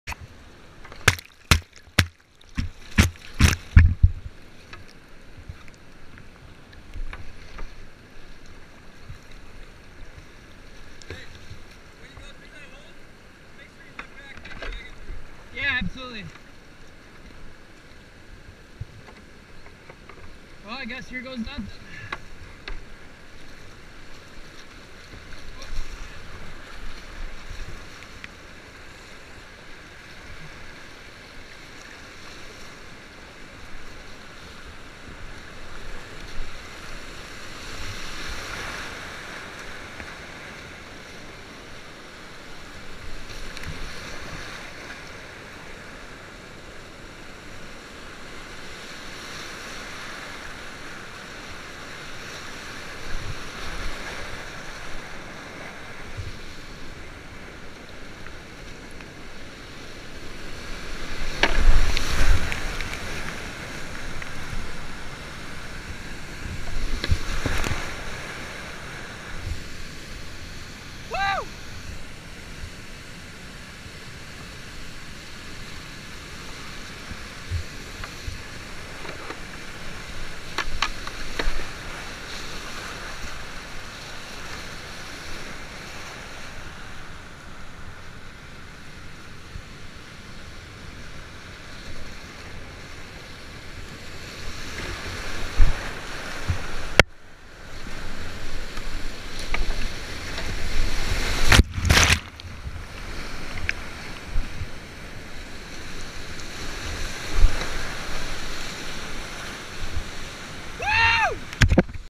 "The Wall" Oldenburg Section, St. Louis River 4000 CFS
Raw GoPro footage of "The Wall" Oldenburg Section, St. Louis River- 4000CFS.